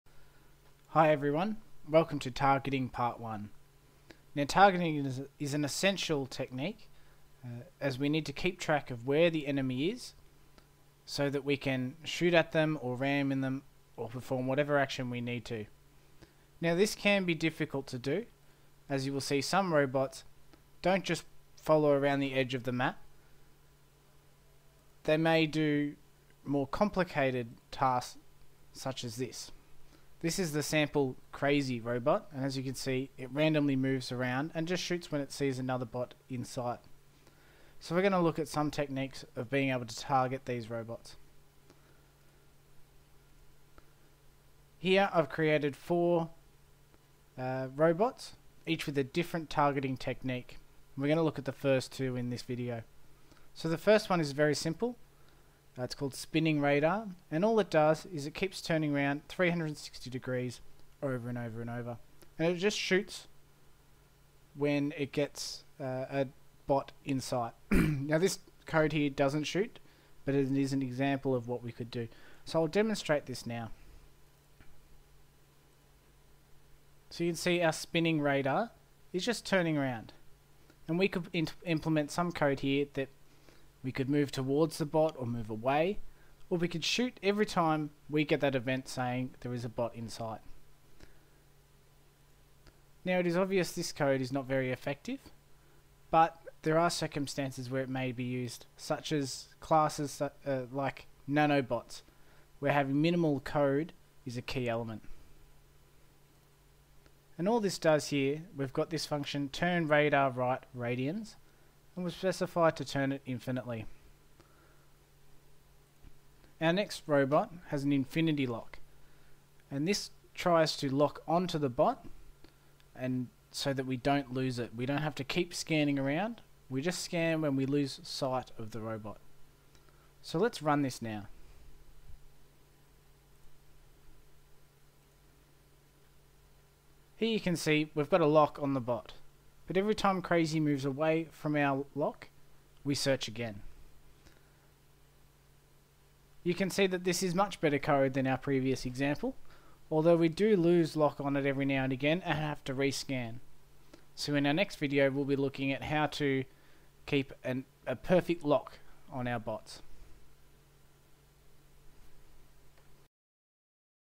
RoboCode Tutorial 8 - Targeting Part 1
Part 8 of 10 videos designed to help introduce new users to RoboCode. This video shows some basic targeting techniques.